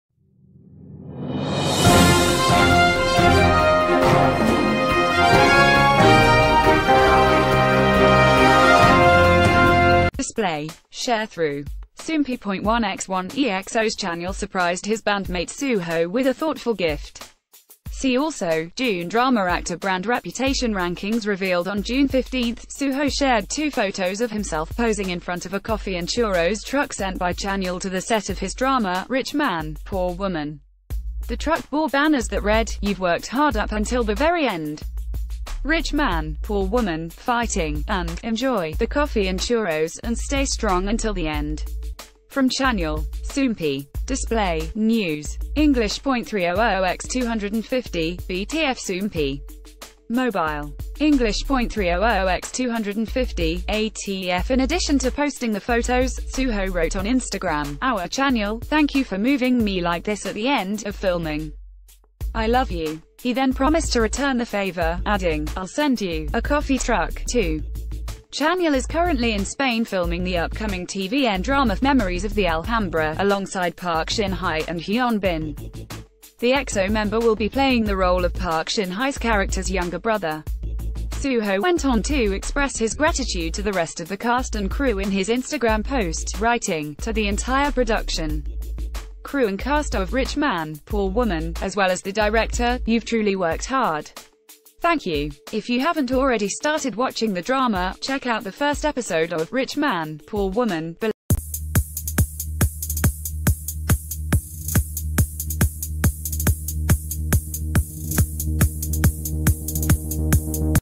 EXO's Chanyeol Surprises Suho With Coffee Truck
Display. Sharethrough. Soompi.1x1 EXO’s Chanyeol surprised his bandmate Suho with a thoughtful gift!...